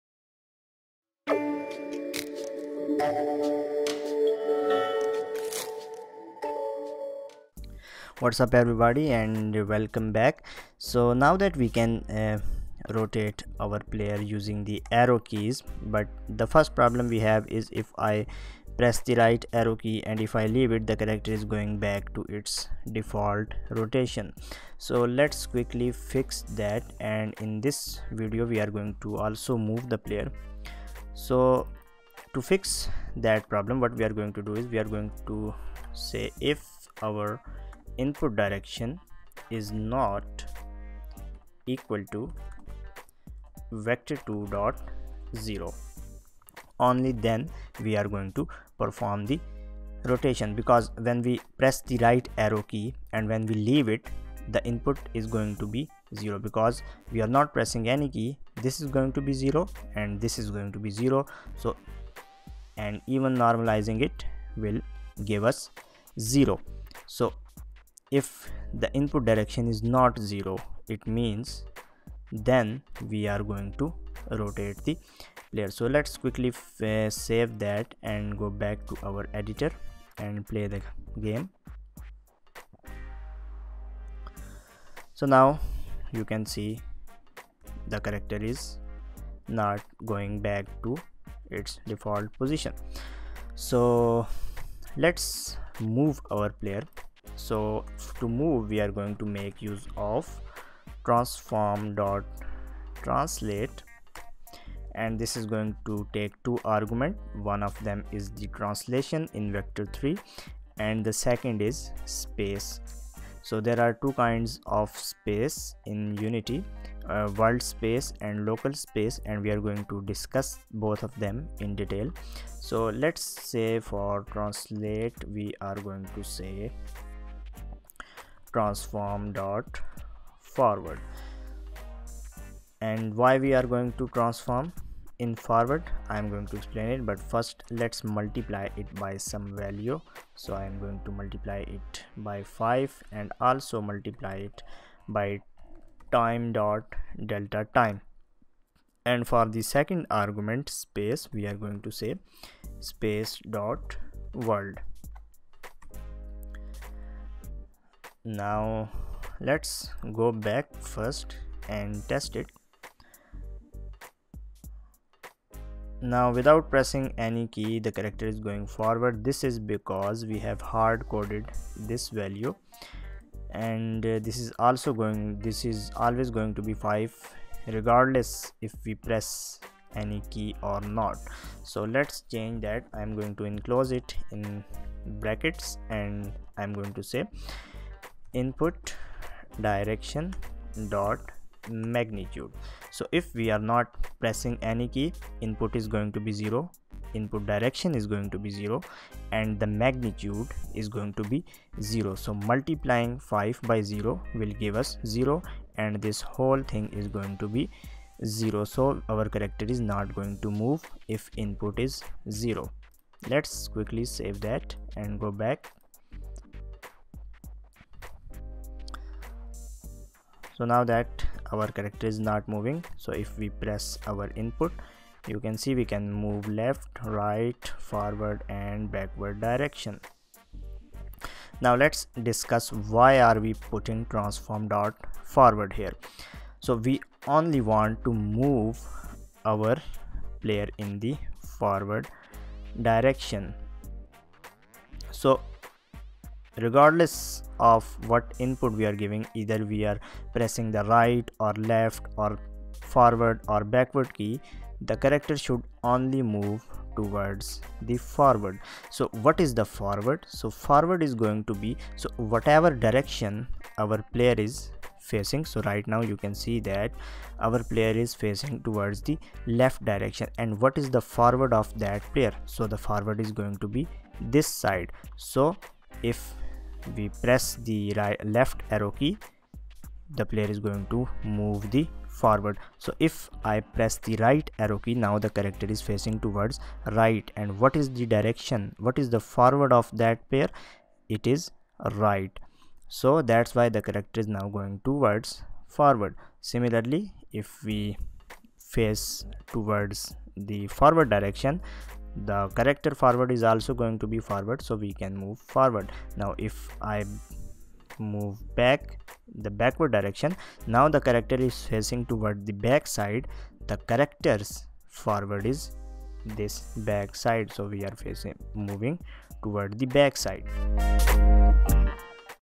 Mobile Third Person Character Controller - Player Movement [05/13]
This is a video tutorial series on create a Third person Character Controller for Mobile devices and PC from scratch.
This series is a part of Udemy Multiplayer Third Person Shooter Course take a look at full course at udemy below.